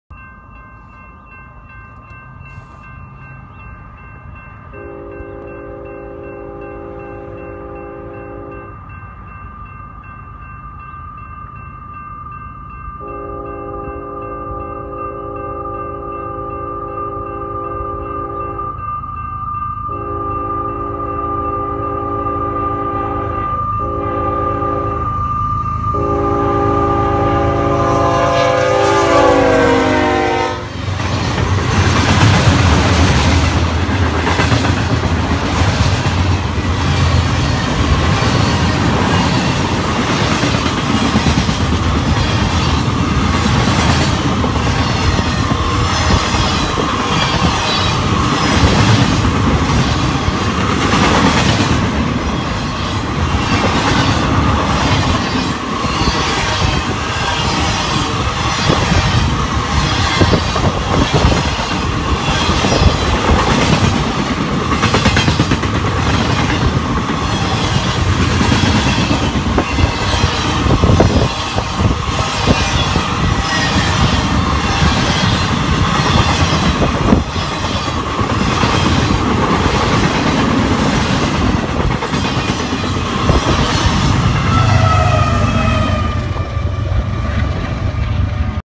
Military Equipment on the move
Jim Krueger caught this train going by up on Jimmy Kerr BLVD in Casa Grande, Arizona